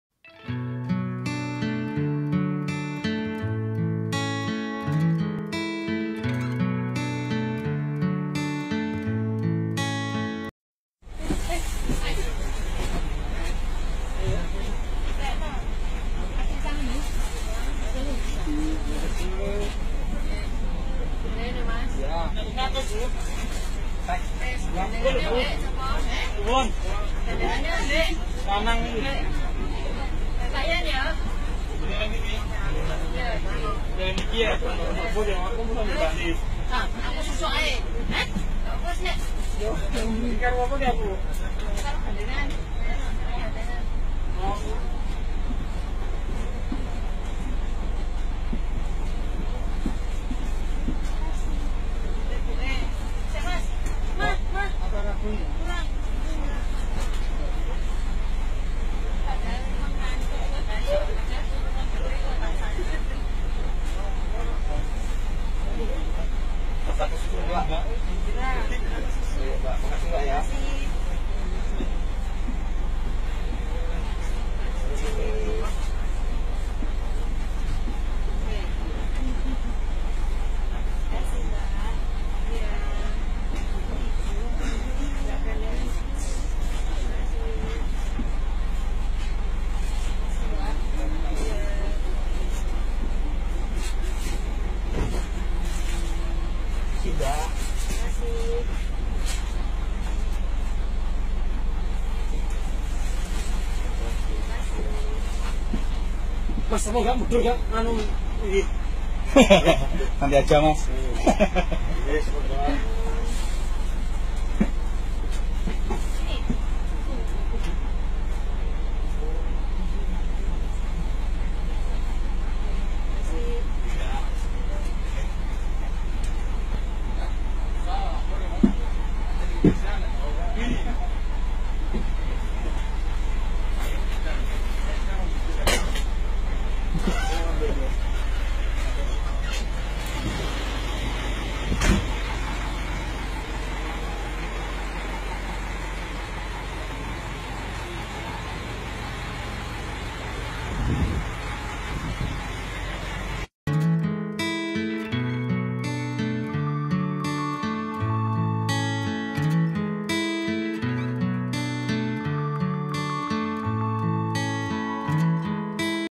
Video hoax terbaru kampreter untuk warga Boyolali
Padahal video tersebut sudah muncul di media satu bulan yang lalu.